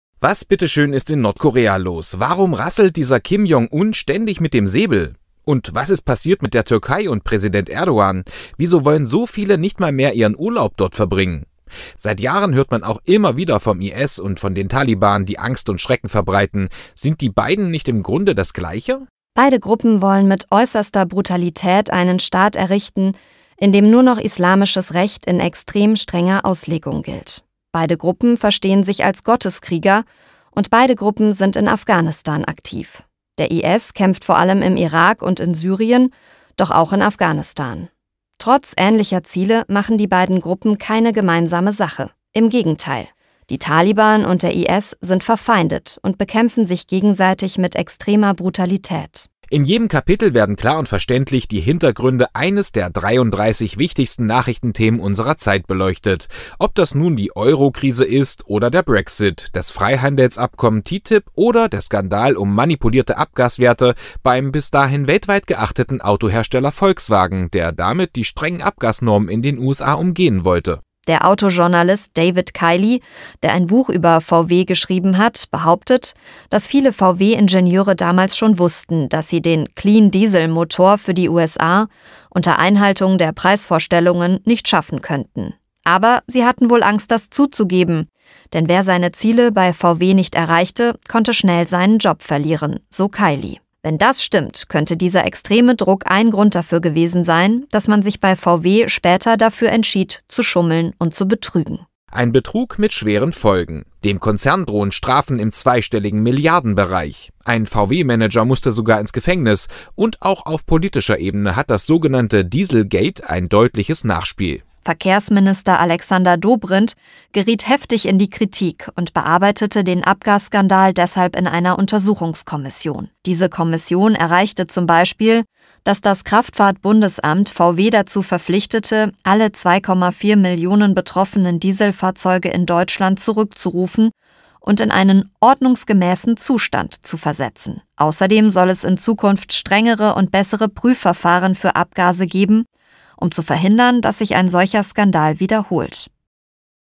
Hörbuch-Tipp der Woche: Ich versteh die Welt nicht mehr
Autor: Tim Schreder, Jennifer Sieglar
Gesprochen von: Tim Schreder, Jennifer Sieglar
Spieldauer: 07 Std. 57 Min.

Der Islamische Staat verbreitet weltweit Angst und Terror, in den USA wird ein Außenseiter zum Präsident gewählt, aus der Türkei vernimmt man ständig neue Schreckensmeldungen über Präsident Erdogan und hierzulande wird die AfD stetig populärer - die Welt der Nachrichten dreht sich immer schneller, dabei sind viele Themen ohne fundiertes Hintergrundwissen kaum zu verstehen. Zugleich konsumieren viele Menschen diese Meldungen vor allem bruchstückhaft über die sozialen Medien.

Hier setzt "Ich versteh die Welt nicht mehr" an und bietet auf verständliche und unterhaltsame Art Hintergründe zu den wichtigsten Nachrichtenthemen unserer Zeit.